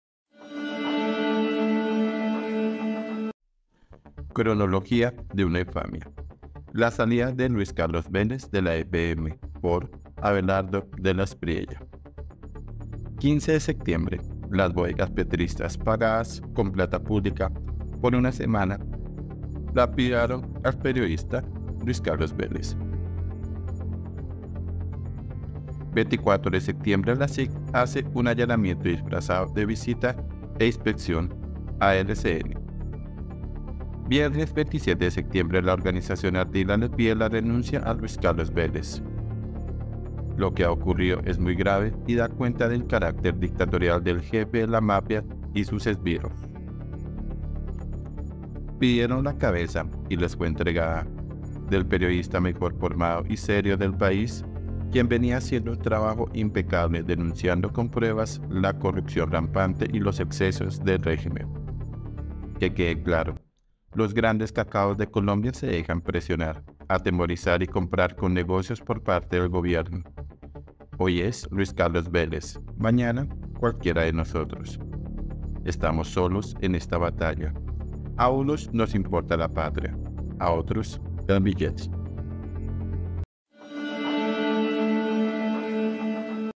CRONOLOGÍA DE UNA INFAMIA. LA SALIDA DE LUIS CARLOS VÉLEZ DE LA FM 📢 Por: Abelardo de la Espriella
Pidieron la cabeza (y les fue entregada) del periodista mejor formado y serio del país, quien venía haciendo un trabajo impecable denunciando con pruebas la corrupción rampante y los excesos del régimen.
columna ABELARDO DE LA ESPRIELLA